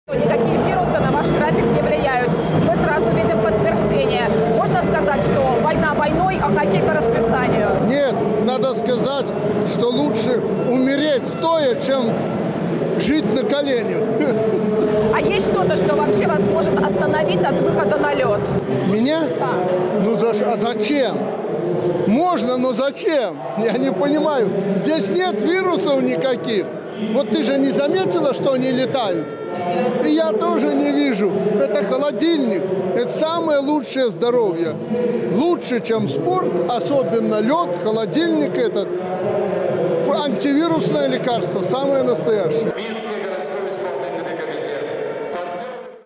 Лукашенко: Хоккей - лучшее антивирусное лекарство!
Хоккейная команда Президента стала первым финалистом тринадцатых республиканских соревнований среди любителей на призы Президентского спортивного клуба.

После второго периода нам удалось задать президенту несколько вопросов.

«Лучше умереть стоя, чем жить на коленях», – так Александр Лукашенко отреагировал на вопрос политического обозревателя ОНТ Анны Пыж, может ли что-то остановить Президента от выхода на лед.

Александр Лукашенко: «Меня? А зачем? Я не понимаю. Здесь нет вирусов никаких! Вот ты же не заметила, что они летают? И я тоже не вижу. Это холодильник, это самое лучшее здоровье! Спорт, особенно лед, - лучшее антивирусное лекарство, самое настоящее!».